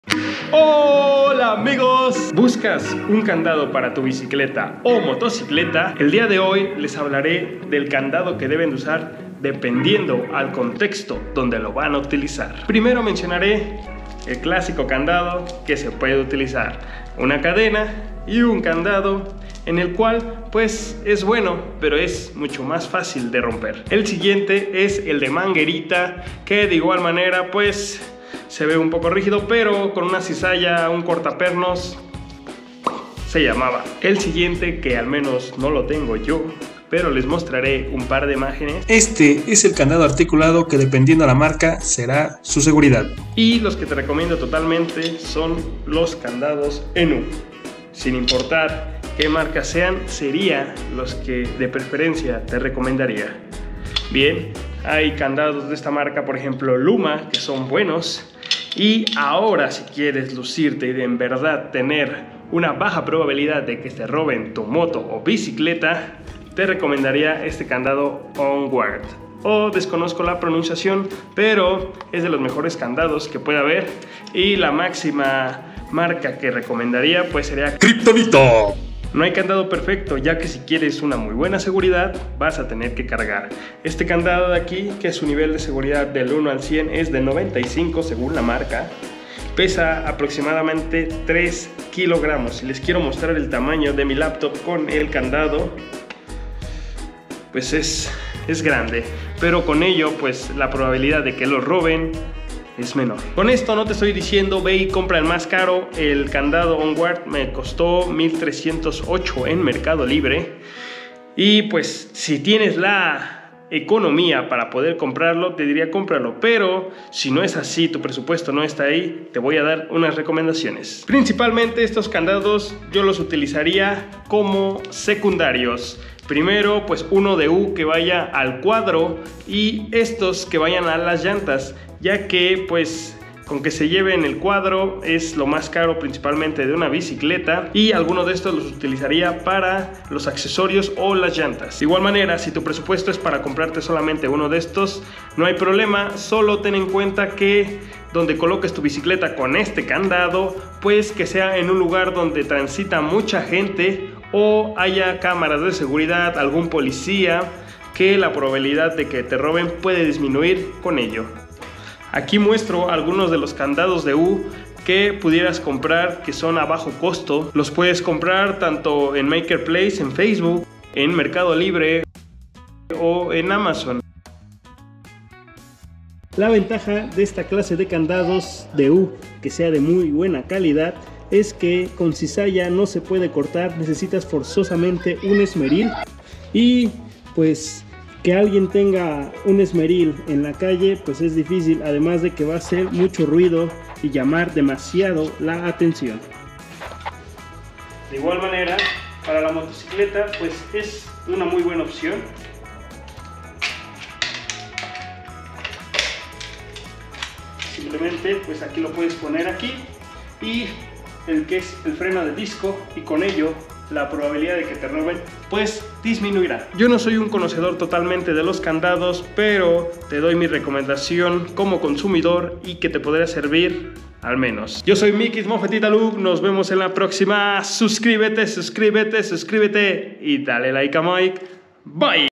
¡Que no te roben! cuida tu bicicleta y motocicleta
Les doy tips para evitar que algun delincuente les robe su bicicleta o motocicleta.